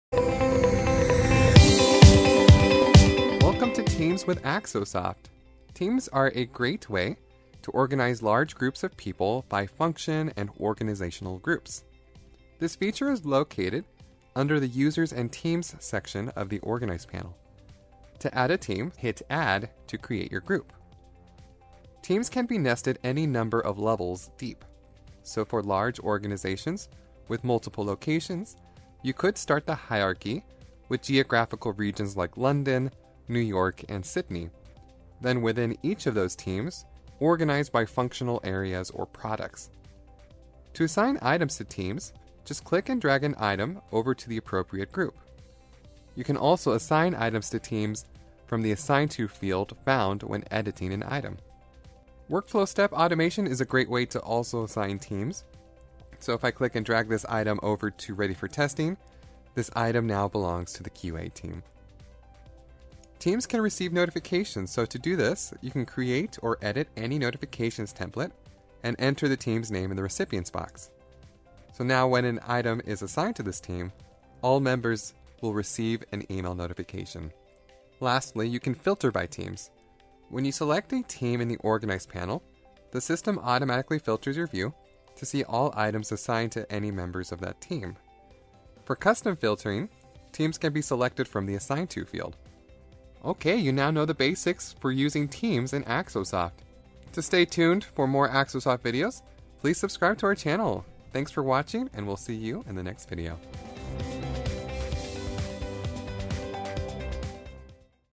Axosoft Quick Start Video #5 - Team Management
Learn all the basics to Axosoft's new team functionality. In this video we talk about how to create teams, assign items to teams, filter teams, plus how to keep them posted.